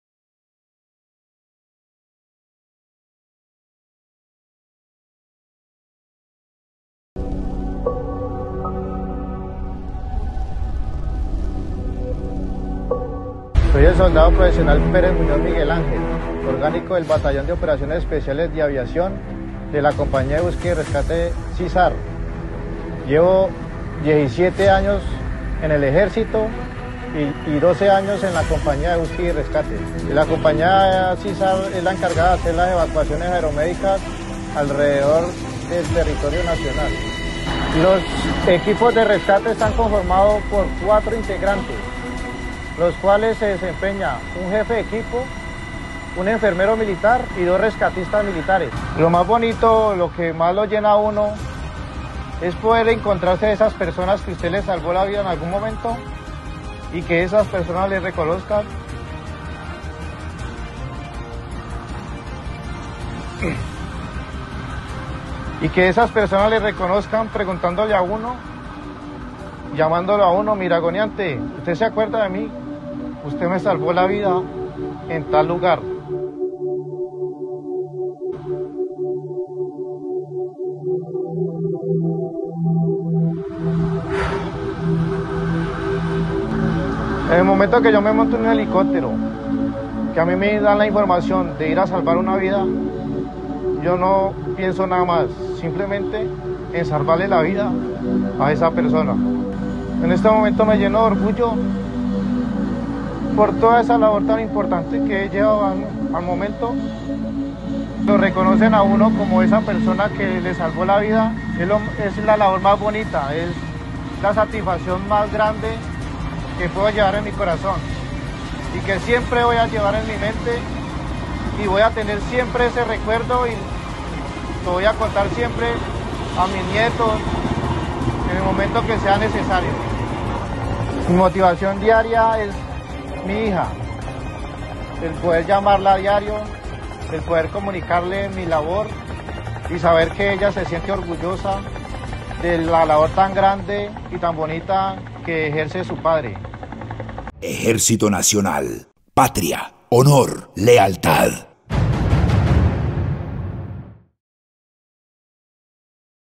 Grupo de combate, búsqueda y rescate, trabajando en las condiciones más peligrosas para salvar vidas
El grupo C-SAR (Combat Search and Rescue) es una unidad especializada de la División de Aviación Asalto Aéreo del Ejército Nacional de Colombia que opera para rescatar a personal civil, militar, pilotos de aeronaves derribadas o heridos en zonas de combate o situaciones peligrosas. Su principal objetivo es recuperar a aquellos en peligro y brindarles atención médica de emergencia si es necesario.

Estos equipos están altamente entrenados y equipados con habilidades de combate, tácticas de rescate, primeros auxilios y supervivencia en condiciones hostiles. Su trabajo puede involucrar operaciones aéreas, terrestres o marítimas, dependiendo de la situación y la ubicación del personal en peligro.